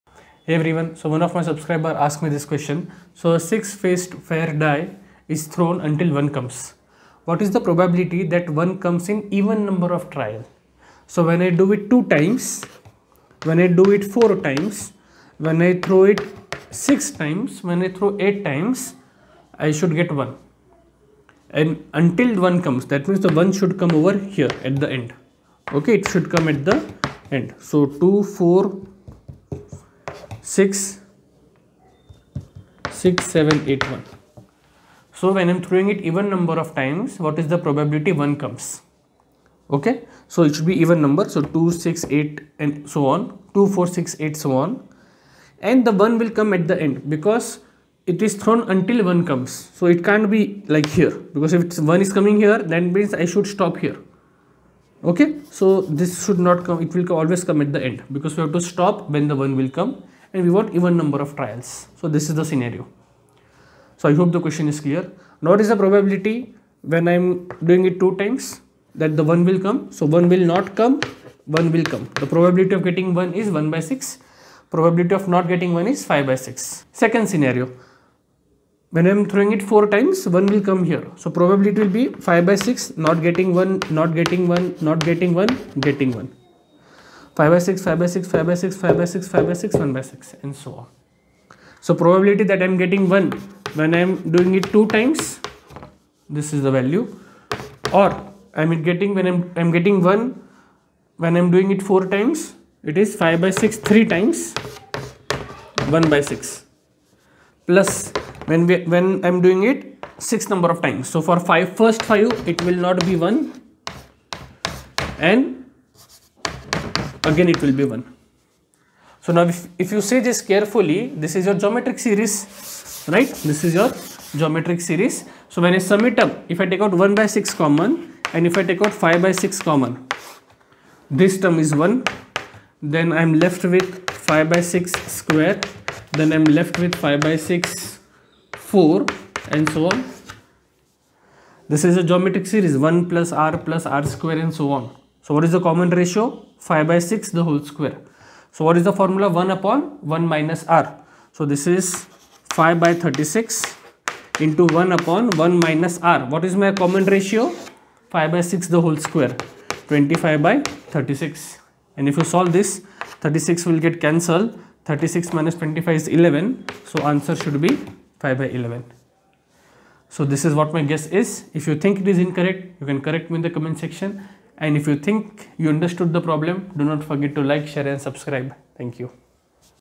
What is the probability of getting 1 at even number of trials?
A six faced fair die is thrown until 1 comes. The probability that one comes in even number of trial is?
Asnwer is very simple. See the video for the answer.